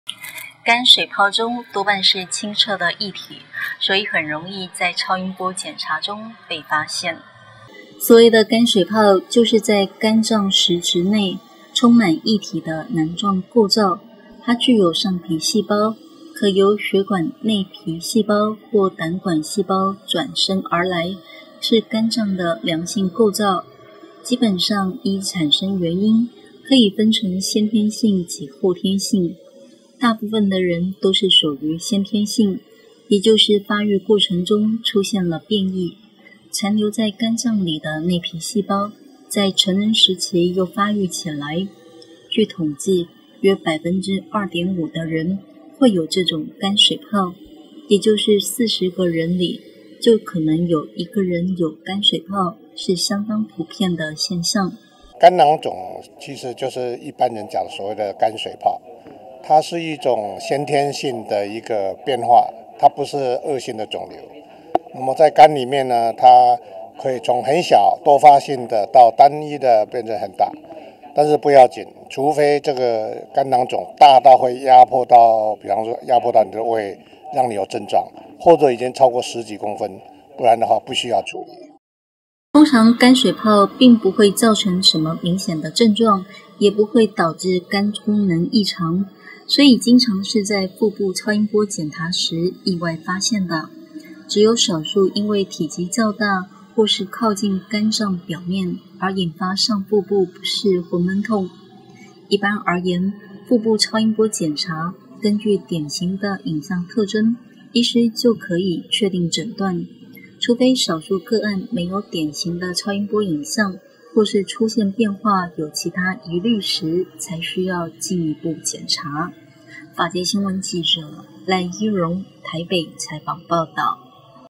法界新聞》20151005沒有明顯症狀的肝水泡
法界新聞》
受訪者 ： 中央研究院院士  廖運範
肝水泡內
多半是清澈的液體
所以在超音波檢查中
很容易被發現